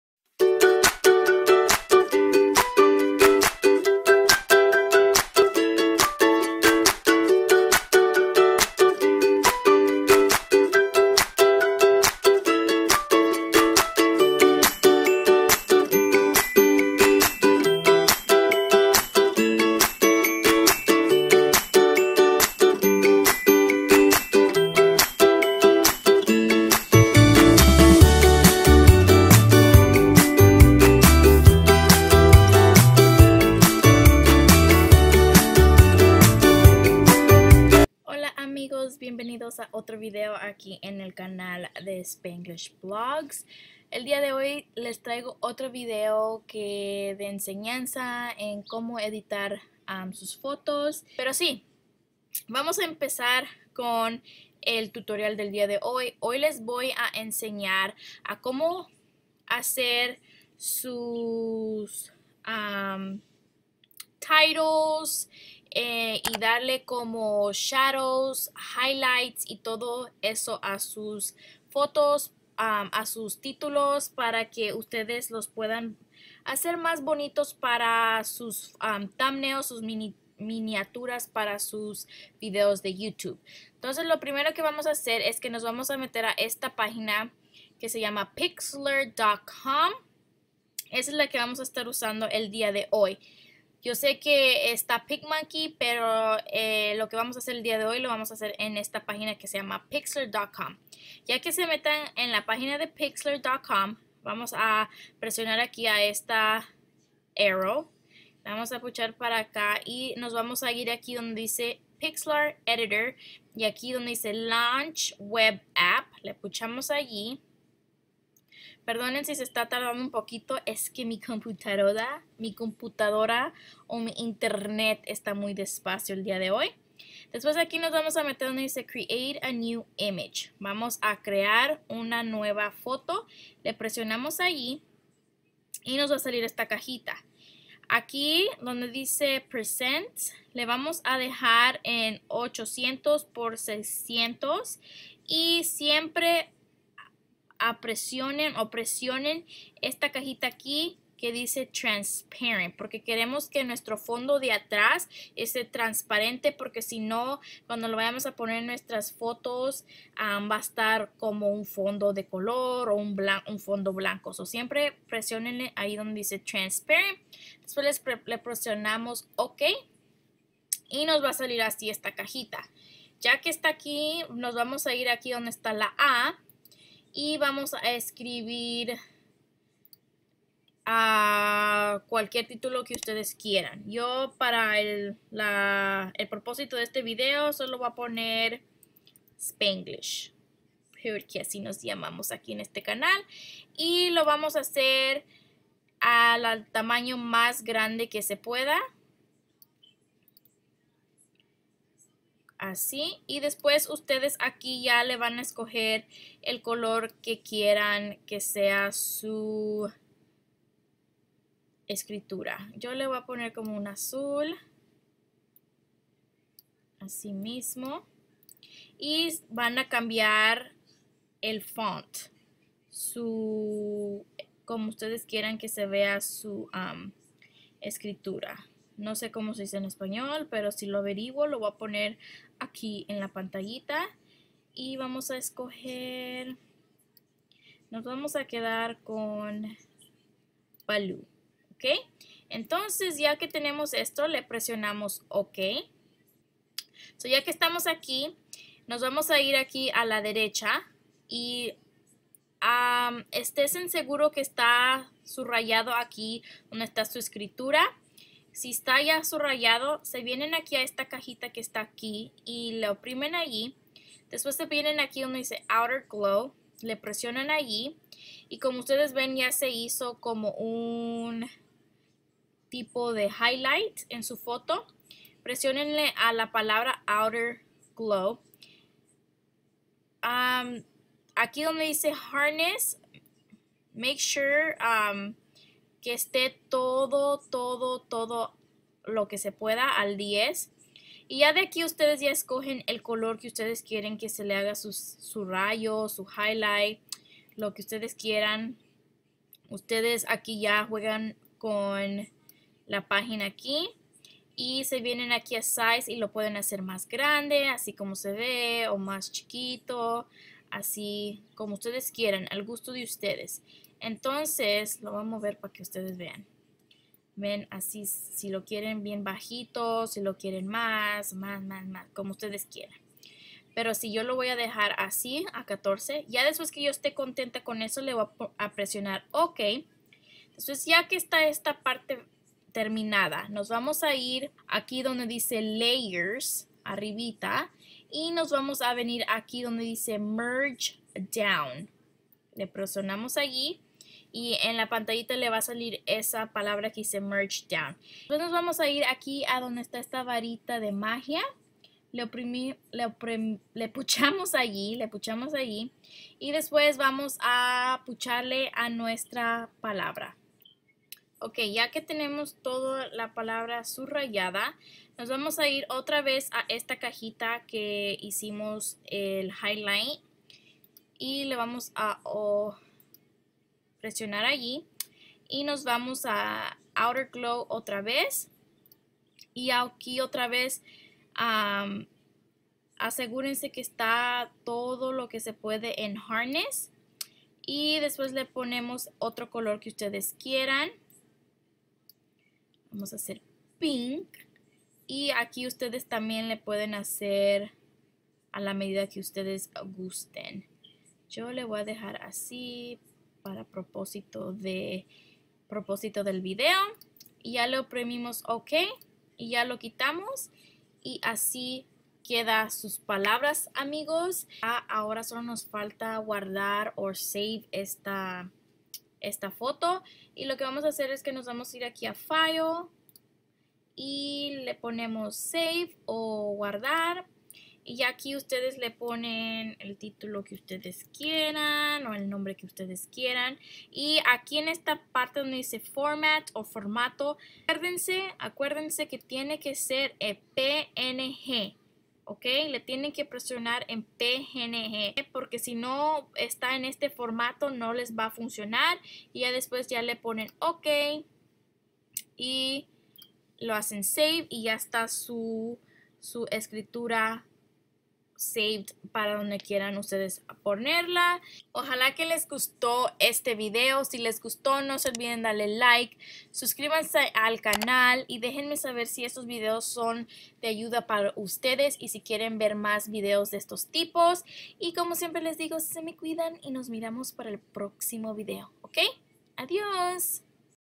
CREAR EFECTOS DE TEXTO CON PIXLR.COM- spanglishvlogs
Feliz Sabado familia spanglish
aqui les tengo otro video de ayuda el video del dia de hoy es en como hacer efectos de 3d a su texto usando la pagina de web pixlr.com
si este tipo de videos les gusta no se olviden en regalarme un like y tambien no se olviden en subscribirse al canal !

Open me!!!

BENDICIONES Y ORALE!

WOUNDERSHARE FILMORA